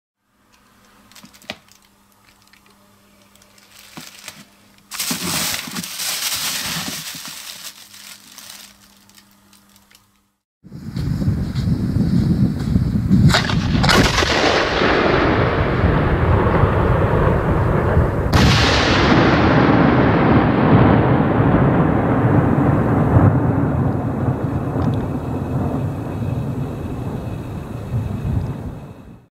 150mm Hydragea | Pyroprodukt
Zündung / Ignition @ 0:11

An sich eine schöne Kugelbombe, in dieser Charge (2016) entgegen dem Etikett jedoch leider ohne Aufstiegseffekte geliefert.

- 6 inch Chinese Shell "Hydragea" from Pyroprodukt

Die im Video gezeigten Artikel wurden legal in der Schweiz durch einen staatlich geprüften Pyrotechniker nach der Berechtigung Feuerwerk B (FWB) erworben und gezündet.